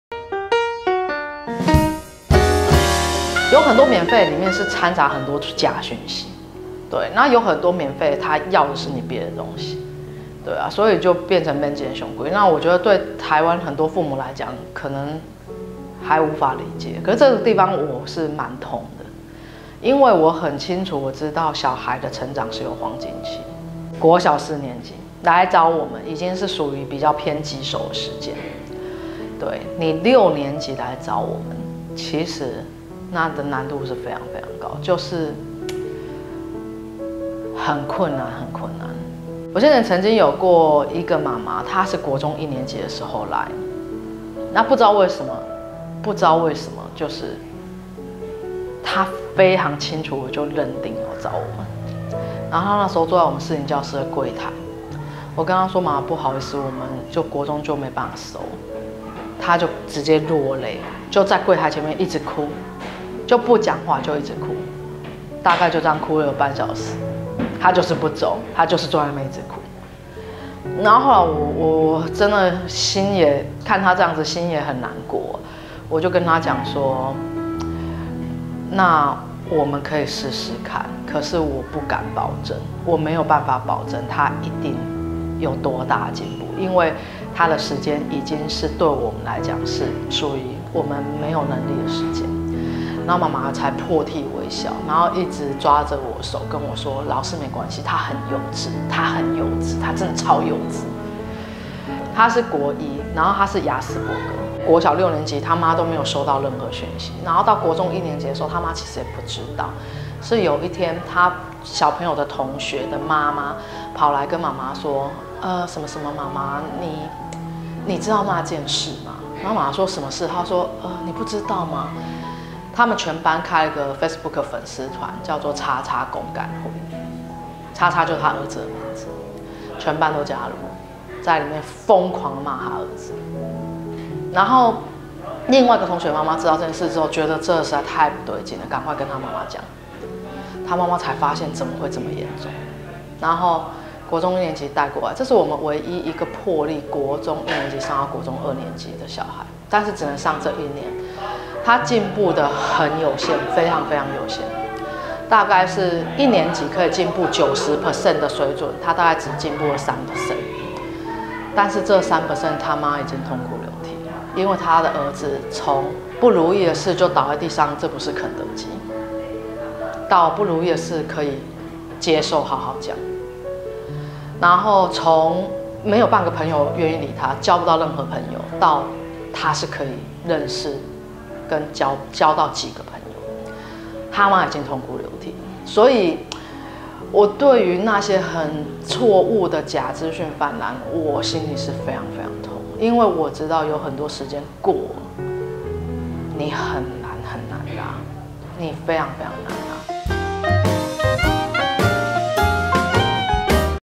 【跳養app現代教養關鍵】小孩的成長是有黃金期的_Part6｜觀賞故事
對於那些很錯誤的假資訊泛濫，我們心裡是非常痛的。
因為我們知道有很多時間過了，你很難很難拉，
雖然對台灣很多父母來講，可能還是很難理解。

但我們很清楚的知道小孩的成長是有黃金期，
國小四年級來找我們已經是屬於比較偏棘手的時間了，
六年級來找我們難度已經非常高⋯⋯。